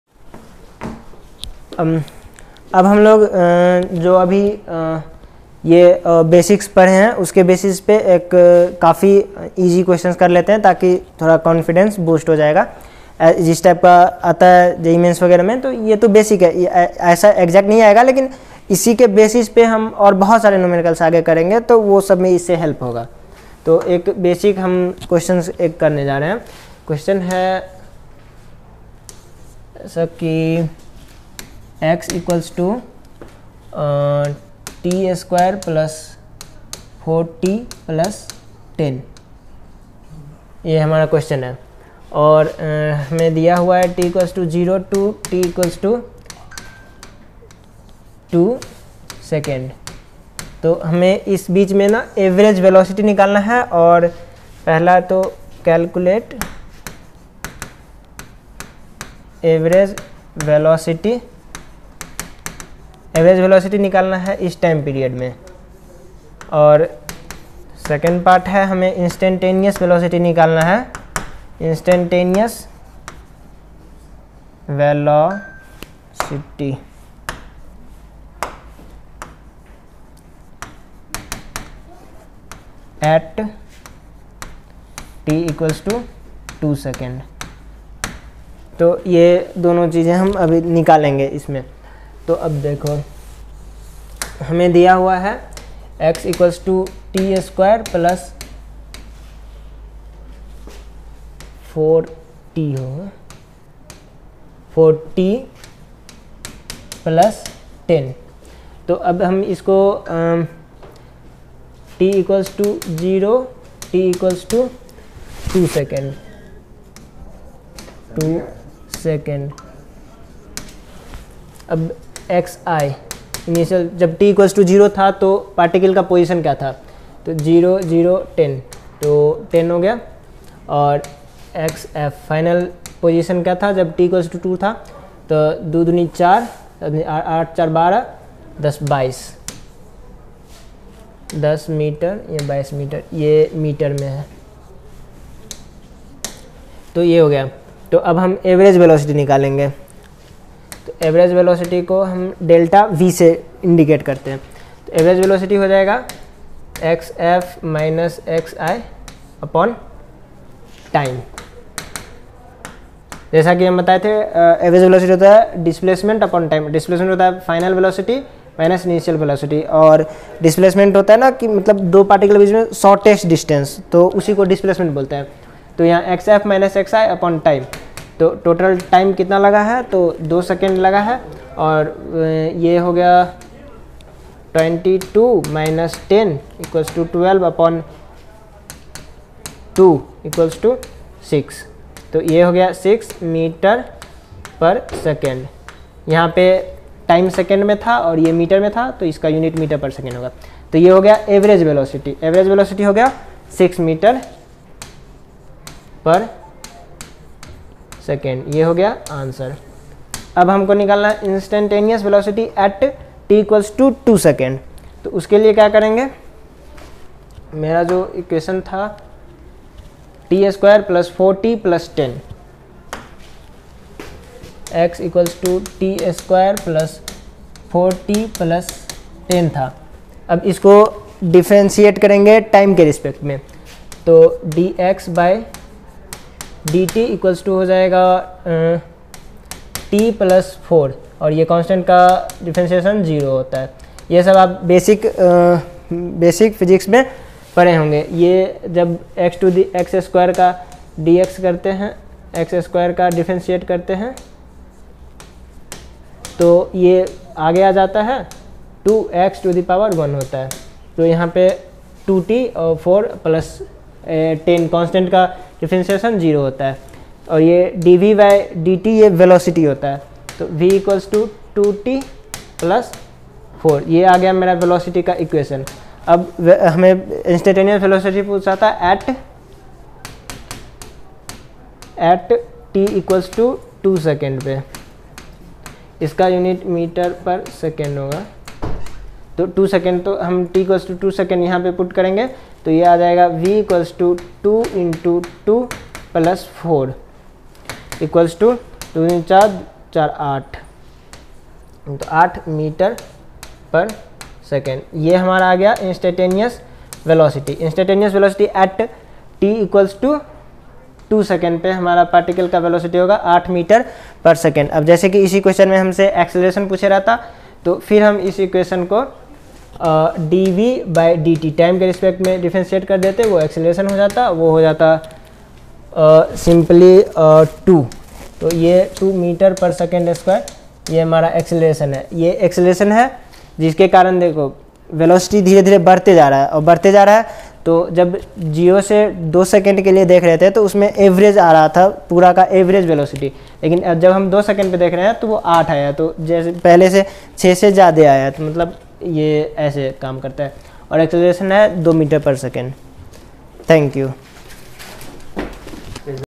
Motion || A simple question based on previous theory || 11th physics || RAVISH KUMAR YADAV
Basics of Mechanics || Derivation of three equations of motion || 11th physics || RAVISH KUMAR YADAV || SR20 ||